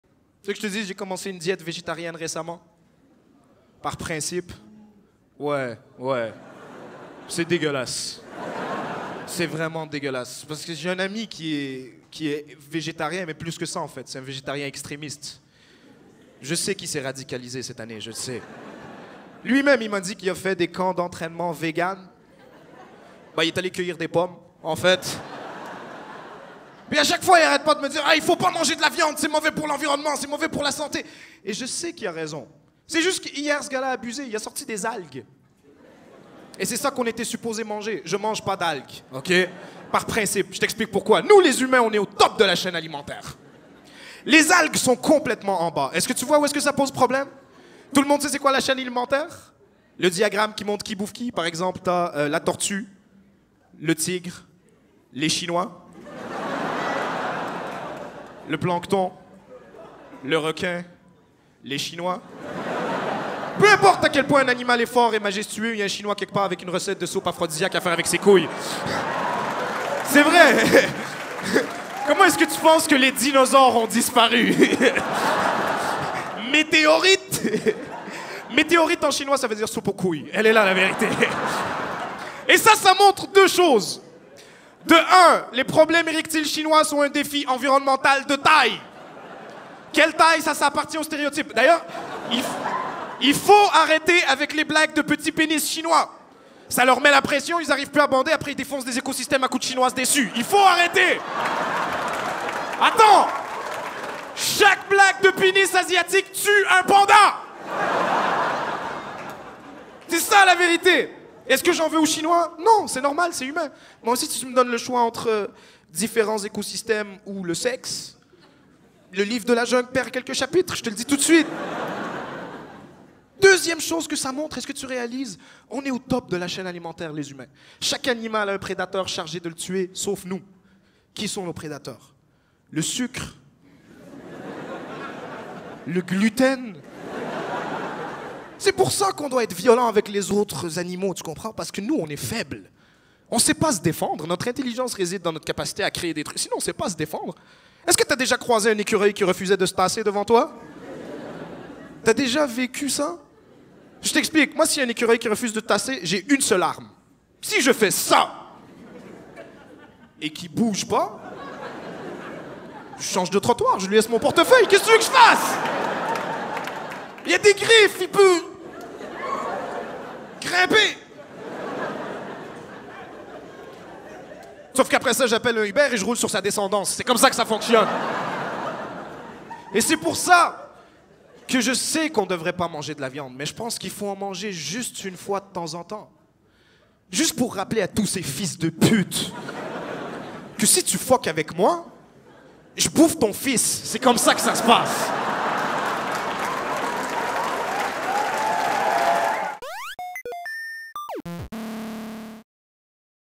La chaîne alimentaire | Anas Hassouna
La chaîne alimentaire expliquée aux végétariens! 🌿🥑

Chaque mercredi 21h à L'Abreuvoir à Montréal!